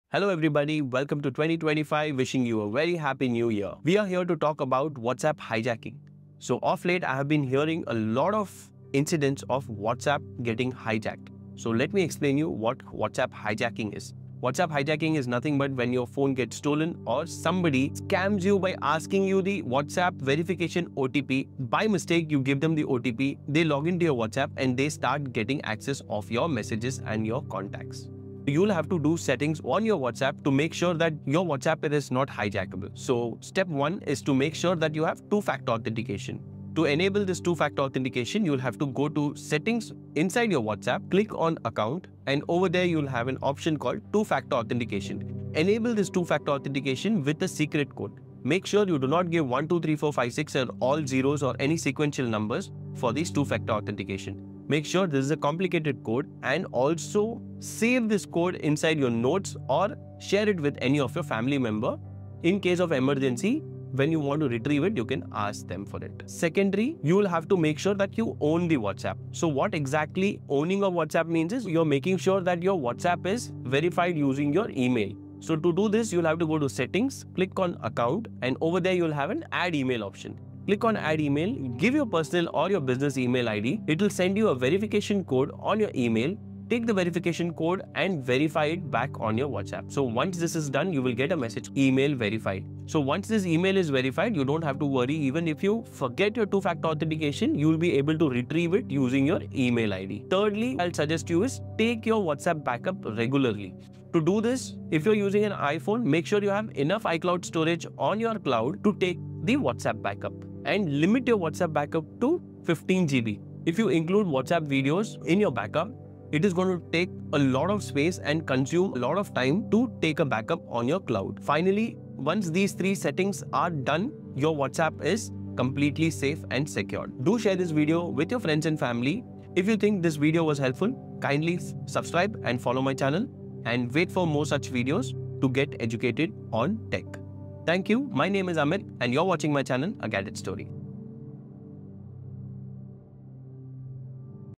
Securing Your WhatsApp Account: Preventing Hijacking and Protecting Your Data.
In this video, we talk about the issue of WhatsApp hijacking and provide steps to secure your WhatsApp account. We discuss enabling two-factor authentication, verifying your WhatsApp with an email, and regularly backing up your WhatsApp data to keep your account safe and secure.